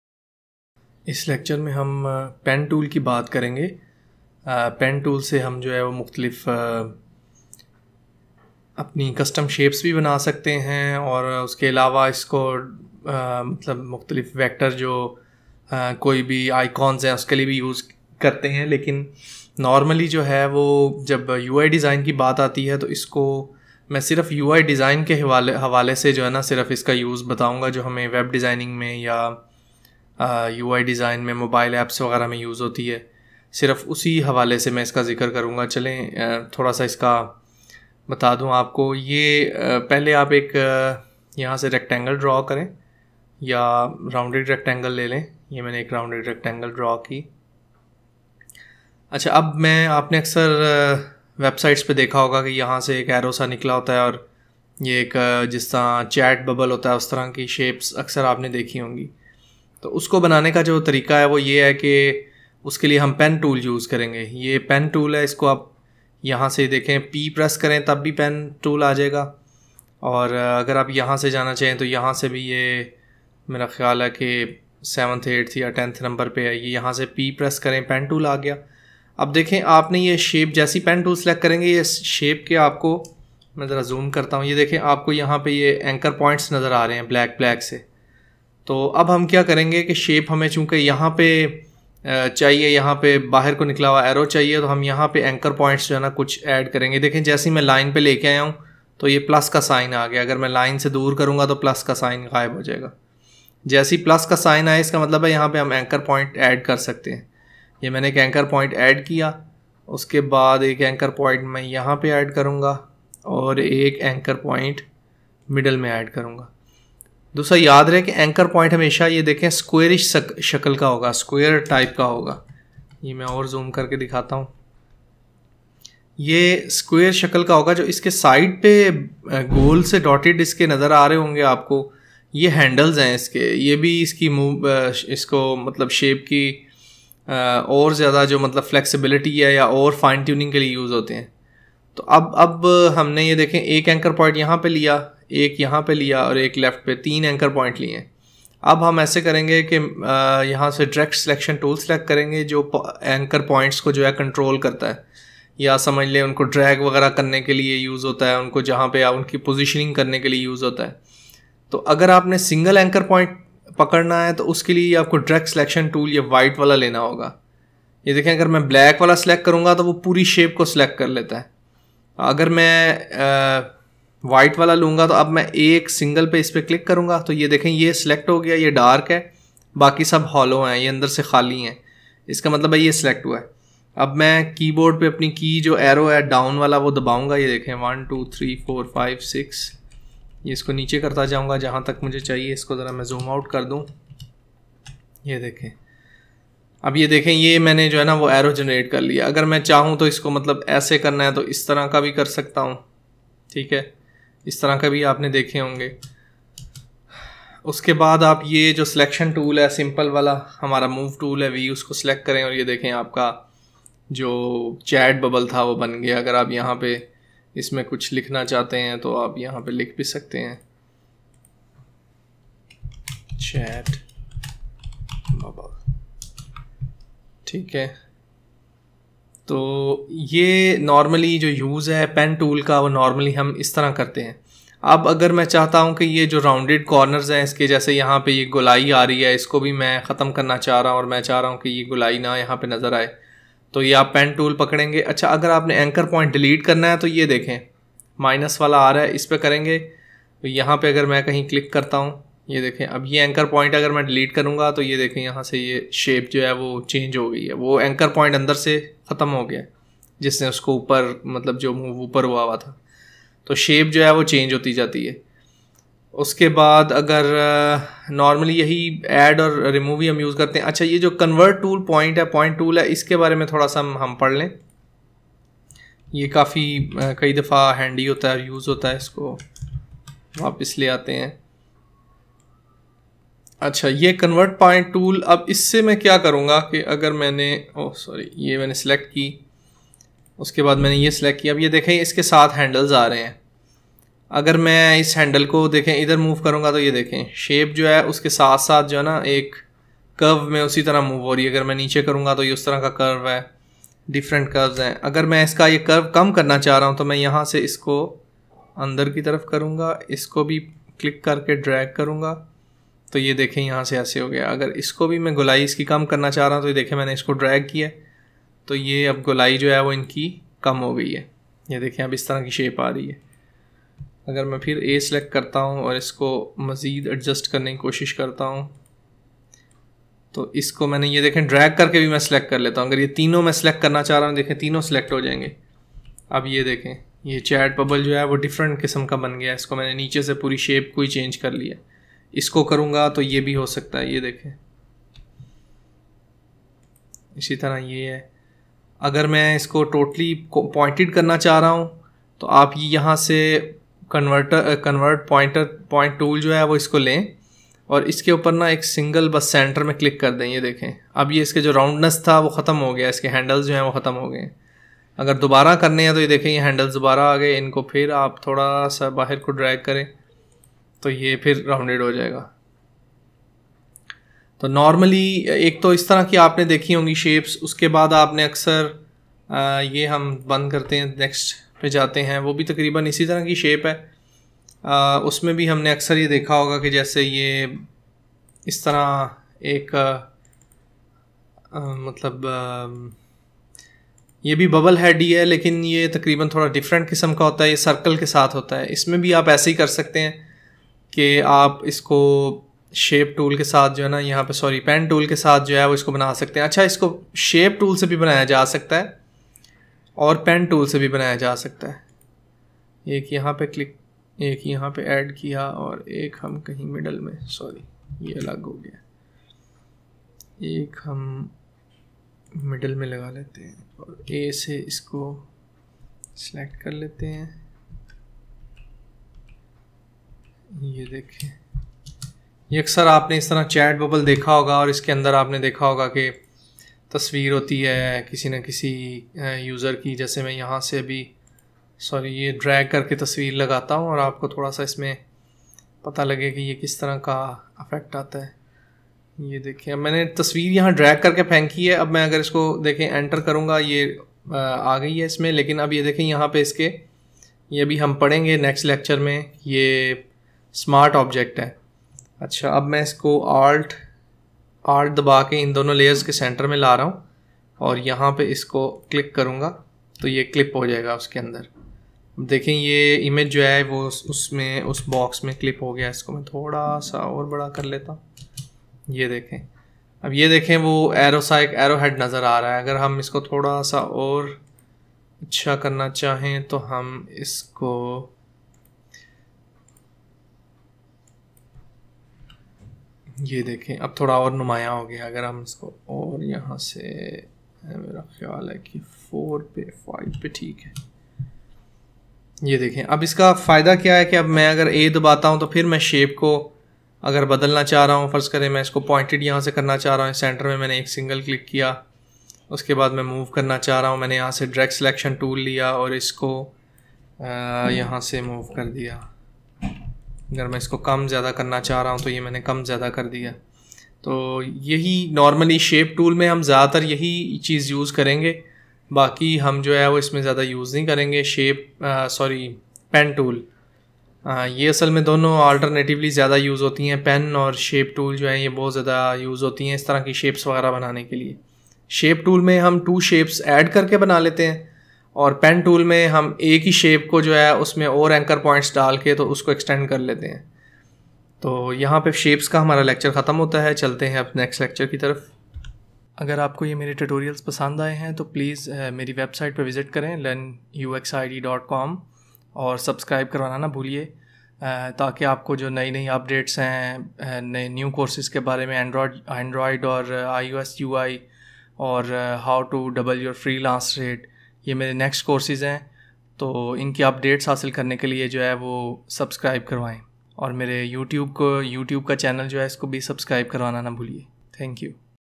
Photoshop Tutorial in Urdu Hindi - Photoshop Pen Tool Lesson13
In this video tutorials, we will learn how to use Photoshop Pen tool for UI Design. I will not go in depth but will touch only things that we use in UI design only situations

17+ Pro UI UX Design Course bundle for $99 (Biannual)